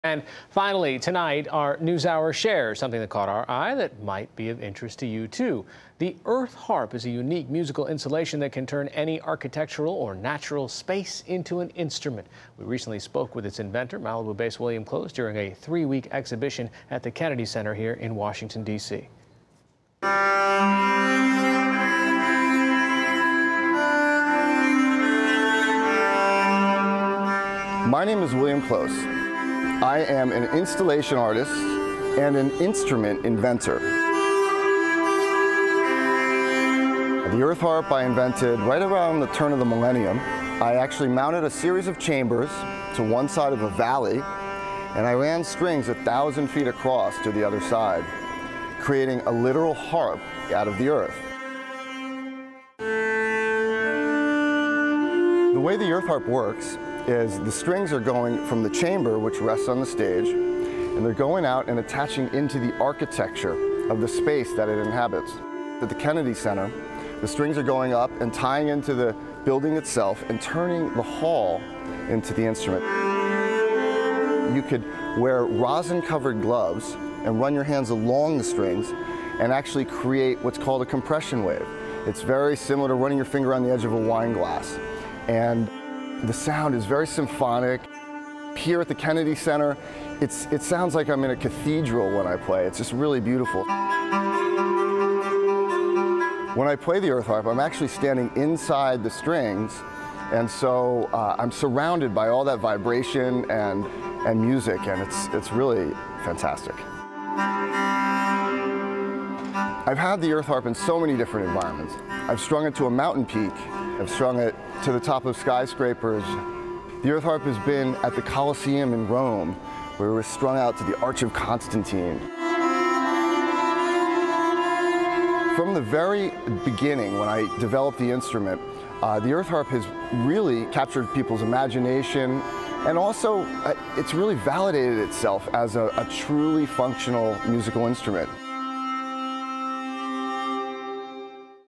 Artist’s ‘earth harp’ a site to behold — and hear
In our NewsHour Shares moment of the day, installation artist William Close is creating a new kind of musical space with his invention, the earth harp. By fixing 1,000-foot strings to mountain peaks, arches, the tops of skyscrapers, canyon walls or the interior of the Kennedy Center, Close can turn any architectural or natural environment into a totally unique instrument.